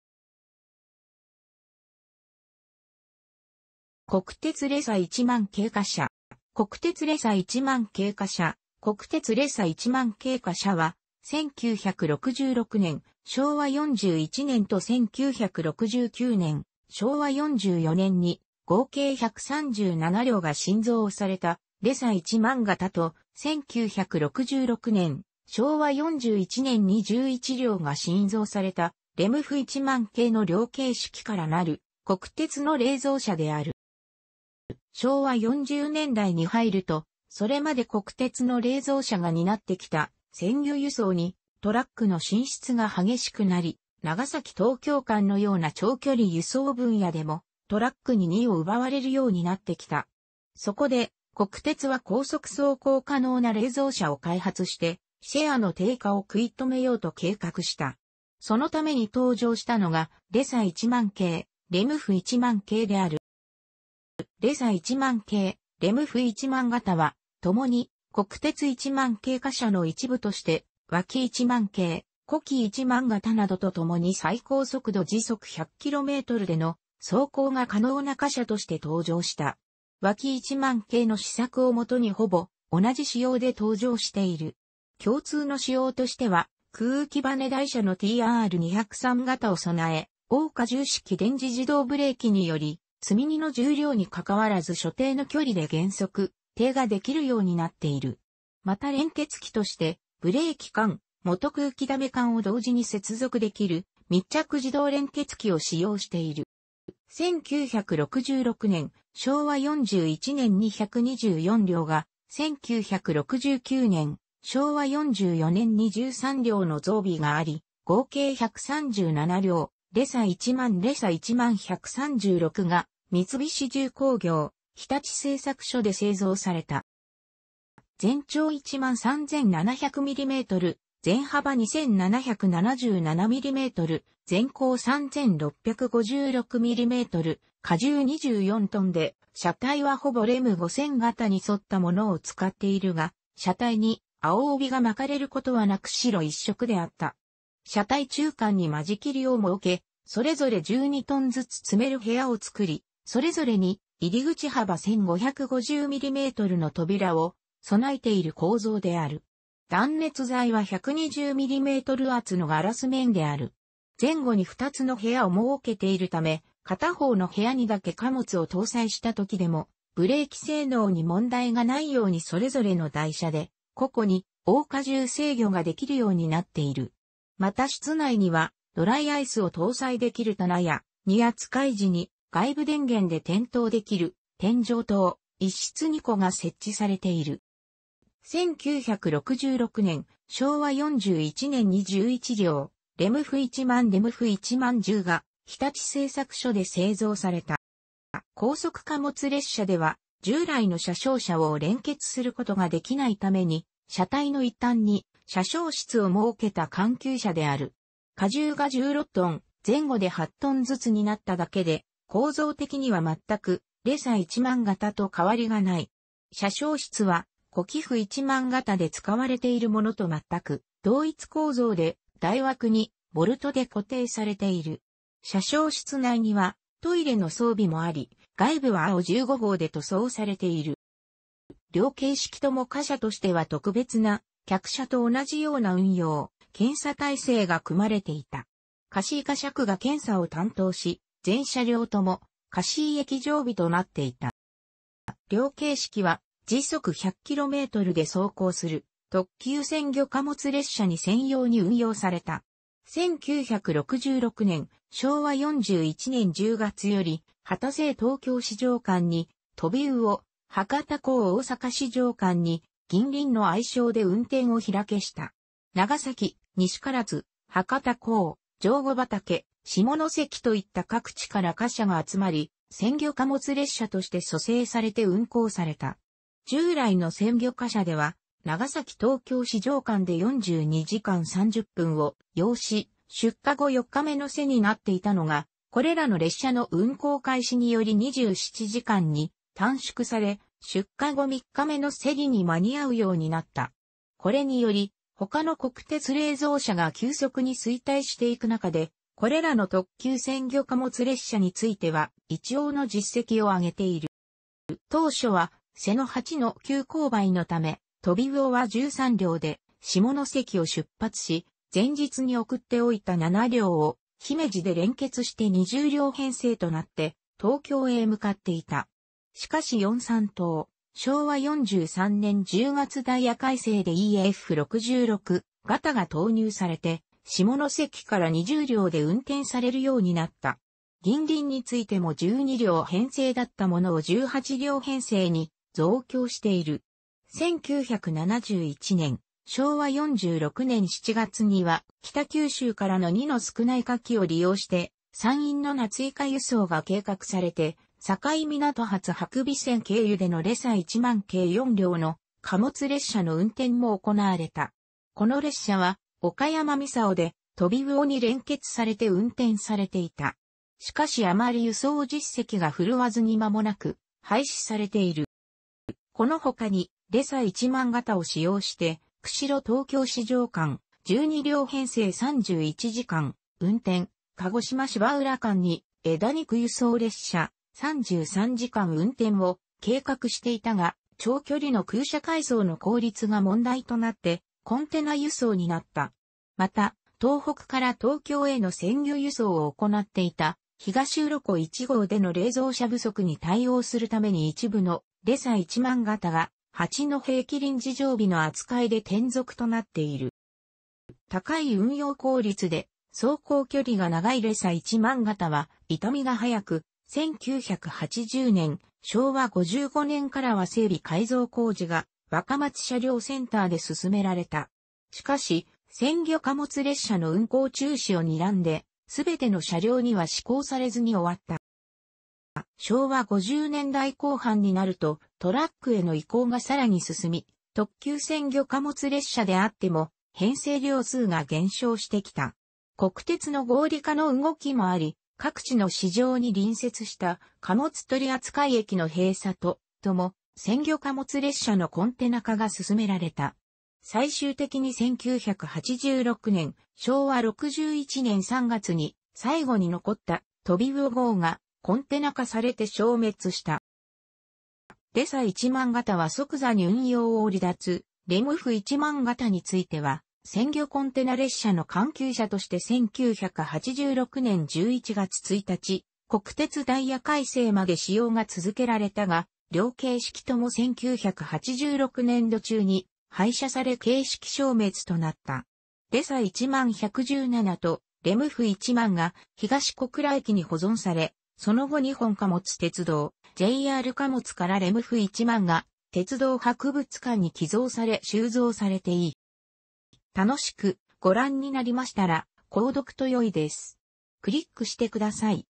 国鉄レサ10000系貨車
国鉄レサ10000系貨車（こくてつレサ10000けいかしゃ）は、1966年（昭和41年）と1969年（昭和44年）に合計137両が新造されたレサ10000形と、1966年（昭和41年）に11両が新造されたレムフ10000形の両形式からなる、国鉄の冷蔵車である。

昭和40年代に入ると、それまで国鉄の冷蔵車が担ってきた鮮魚輸送にトラックの進出が激しくなり、長崎 - 東京間のような長距離輸送分野でもトラックに荷を奪われるようになってきた。そこで国鉄は高速走行可能な冷蔵車を開発してシェアの低下を食い止めようと計画した。そのために登場したのがレサ10000形、レムフ10000形である。

レサ10000形・レムフ10000形は、共に国鉄10000系貨車の一部として、ワキ10000形・コキ10000形などとともに最高速度 100 km/hでの走行が可能な貨車として登場した。ワキ10000形の試作を元にほぼ同じ仕様で登場している。共通の仕様としては、空気バネ台車のTR203形を備え、応荷重式電磁自動ブレーキにより積み荷の重量に関わらず所定の距離で減速・停止ができるようになっている。また連結器としてブレーキ管・元空気ダメ管を同時に接続できる密着自動連結器を使用している。

1966年（昭和41年）に124両が、1969年（昭和44年）に13両の増備があり合計137両（レサ10000 - レサ10136）が三菱重工業・日立製作所で製造された。

全長13,700mm、全幅2,777mm、全高3,656mm、荷重24tで、車体はほぼレム5000形に沿ったものを使っているが、車体に青帯が巻かれる事はなく白一色であった。車体中間に間仕切りを設け、それぞれ12tずつ積める部屋を作り、それぞれに入口幅1,550mmの扉を備えている構造である。断熱材は120mm厚のガラス綿である。前後に2つの部屋を設けているため、片方の部屋にだけ貨物を搭載した時でもブレーキ性能に問題がないようにそれぞれの台車で個々に応荷重制御ができるようになっている。また室内にはドライアイスを搭載できる棚や、荷扱い時に外部電源で点灯できる天井灯（1室2個）が設置されている。

1966年（昭和41年）に11両（レムフ10000 - レムフ10010）が日立製作所で製造された。

高速貨物列車では従来の車掌車を連結することができないために、車体の一端に車掌室を設けた緩急車である。荷重が16t（前後で8tずつ）になっただけで、構造的には全くレサ10000形と変わりがない。車掌室は、コキフ10000形で使われているものと全く同一構造で、台枠にボルトで固定されている。車掌室内にはトイレの装備もあり、外部は青15号で塗装されている。

両形式とも貨車としては特別な、客車と同じような運用・検査体制が組まれていた。香椎貨車区が検査を担当し、全車両とも香椎駅常備となっていた。

両形式は、100km/hで走行する特急鮮魚貨物列車に専用に運用された。1966年（昭和41年）10月より、幡生 - 東京市場間に「とびうお」、博多港 - 大阪市場間に「ぎんりん」の愛称で運転を開始した。長崎、西唐津、博多港、上戸畑、下関といった各地から貨車が集まり鮮魚貨物列車として組成されて運行された。従来の鮮魚貨車では長崎 - 東京市場間で42時間30分を要し出荷後4日目のセリになっていたのが、これらの列車の運行開始により27時間に短縮され、出荷後3日目のセリに間に合うようになった。これにより、他の国鉄冷蔵車が急速に衰退していく中で、これらの特急鮮魚貨物列車については一応の実績を上げている。

当初は瀬野八の急勾配のため、「とびうお」は13両で下関を出発し、前日に送っておいた7両を姫路で連結して20両編成となって東京へ向かっていた。しかしヨンサントオ（昭和43年10月ダイヤ改正）でEF66形が投入されて下関から20両で運転されるようになった。「ぎんりん」についても12両編成だったものを18両編成に増強している。1971年（昭和46年）7月には、北九州からの荷の少ない夏期を利用して山陰の夏イカ輸送が計画されて、境港発伯備線経由でのレサ10000形4両の貨物列車の運転も行われた。この列車は岡山操で「とびうお」に連結されて運転されていた。しかしあまり輸送実績が振るわずにまもなく廃止されている。

この他に、レサ10000形を使用して釧路 - ...